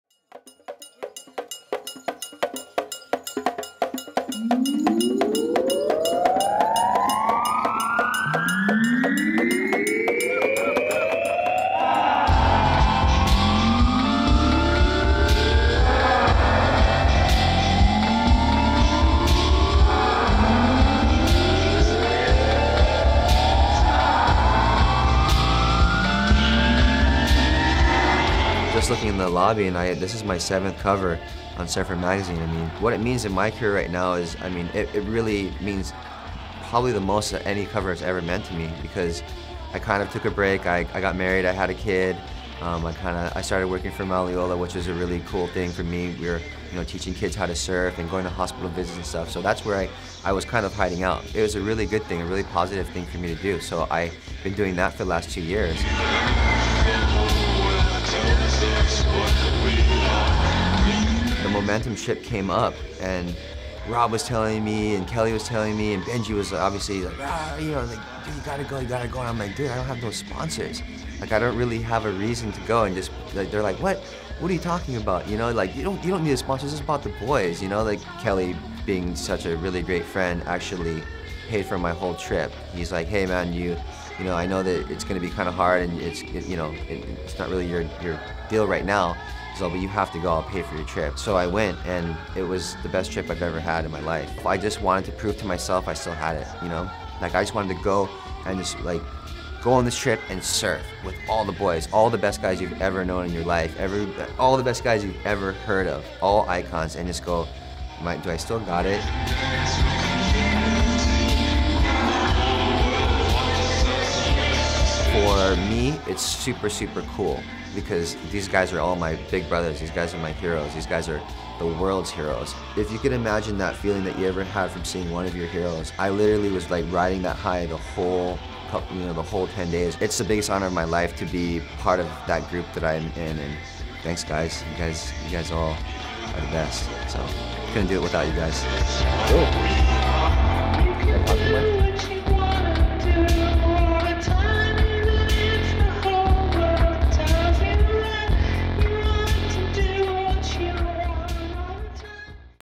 SURFER - Behind the Cover - Kalani Robb
Kalani Robb reacts to being back on the cover of SURFER Magazine.